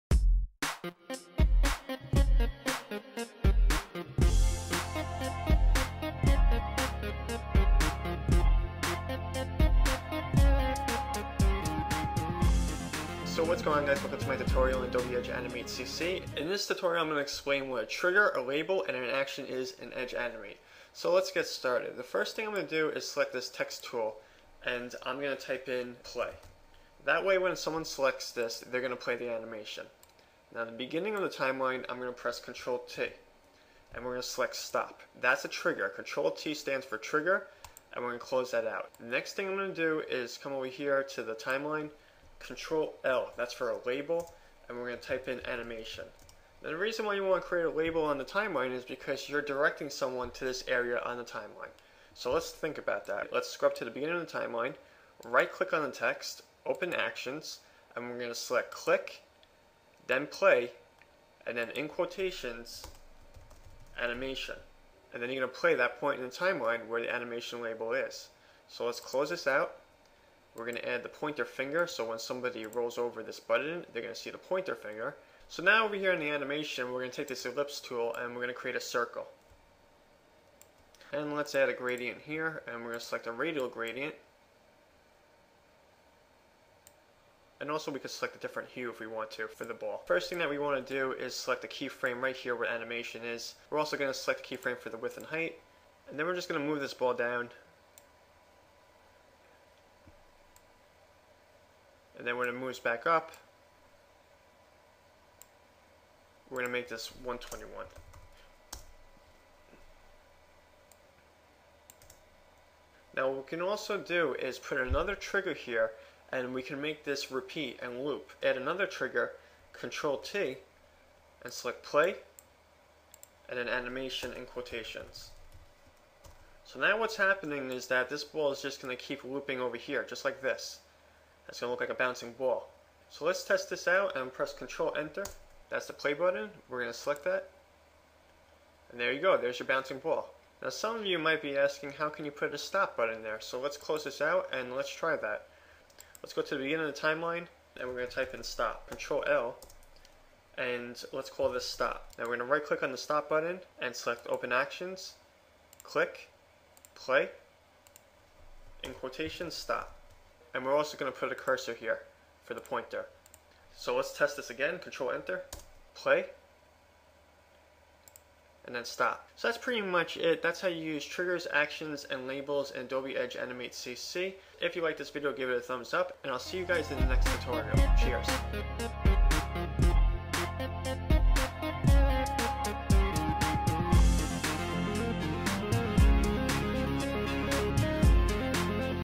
Adobe Edge Animate Tutorial: How To Animate With A Stop and Play Button
In this tutorial I explain how to animate with a stop and play button in Adobe Edge Animate. This tutorial explains how to use triggers,actions, and labels.